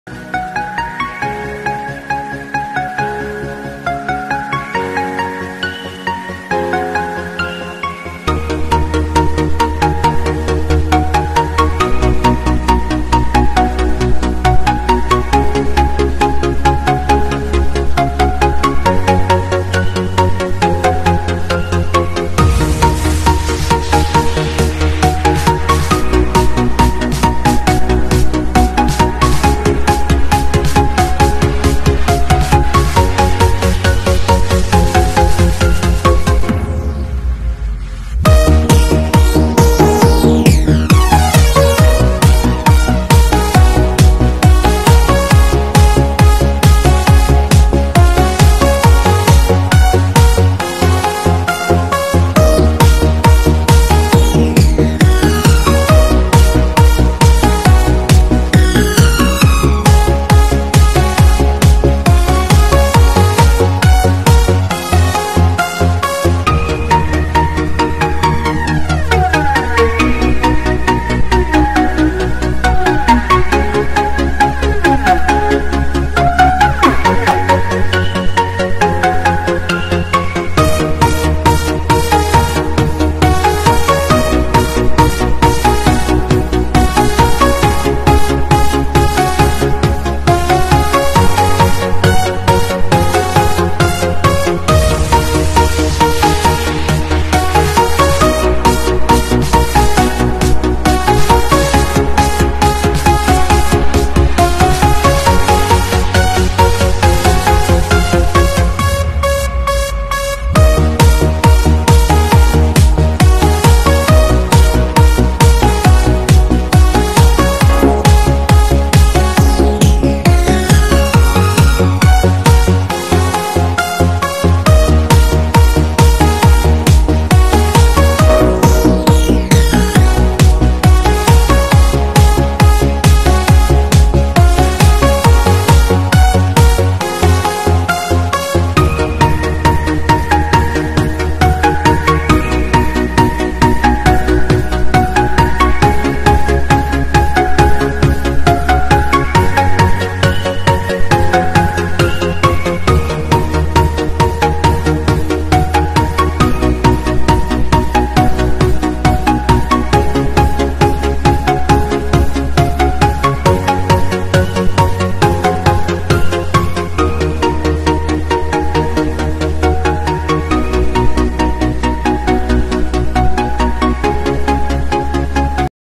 PVD coating process from ultrasonic cleaning to coating
In thie video, you can see the whole PVD coating process : Raw products ultrasonic cleaning by Ultrasonic cleaning machine ---baking by oven---pvd vacuum coating ---products package
Mr Donson Zhang (CICEL sales Director)